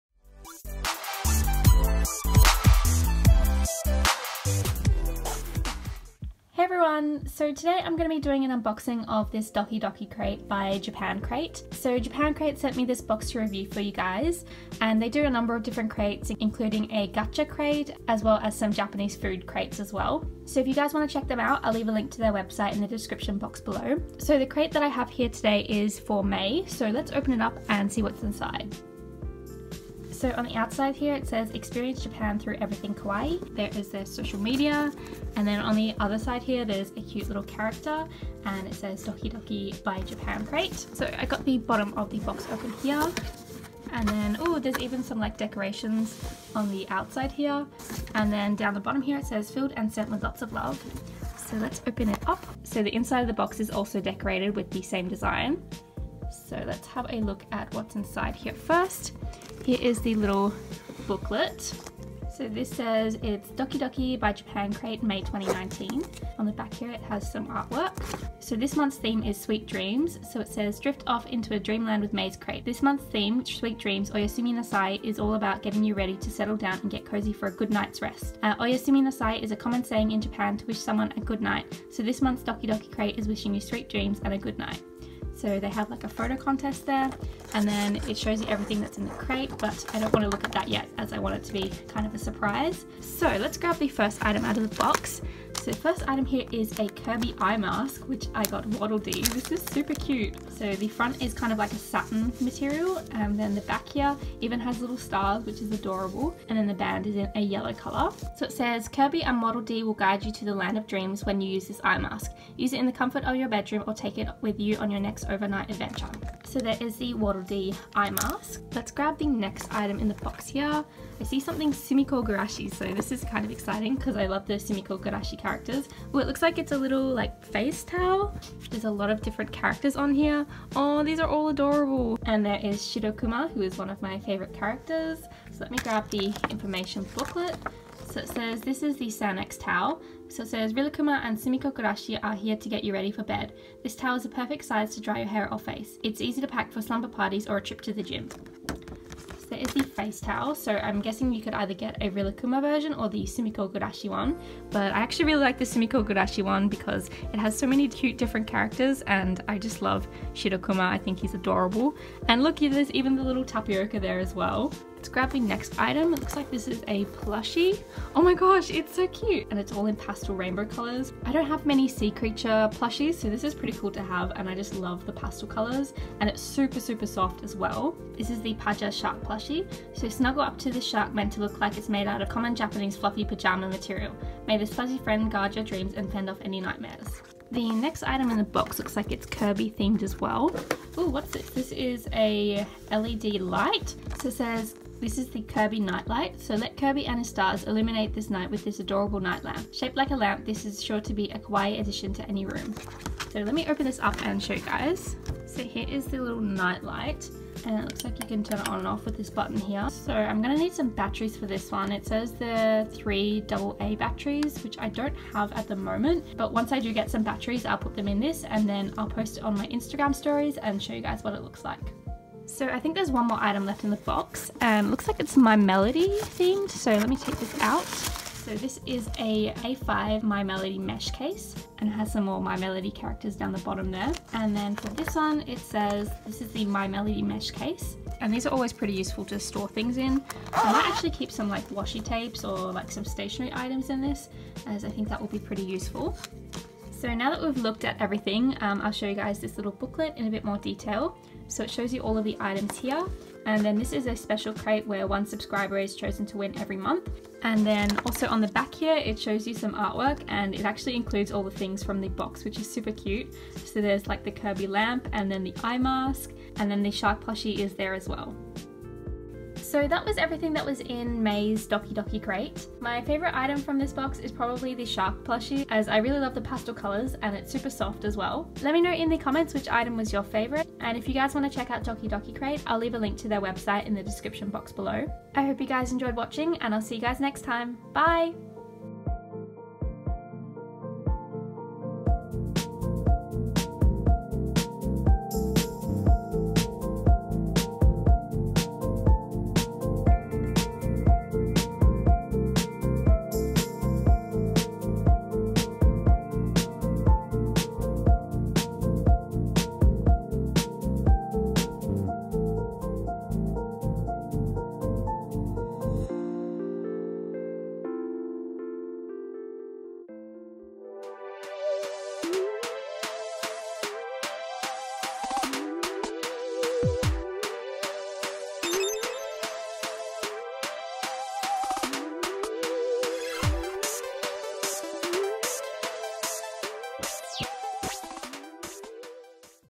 KIRBY, MY MELODY, CUTE PLUSHIES + MORE! | Doki Doki Crate Unboxing ♡
Here is an unboxing of May's Doki Doki Crate that was sent to me for review by Japan Crate! This month's theme was Sweet Dreams and it featured some Kirby, My Melody and other adorable items!!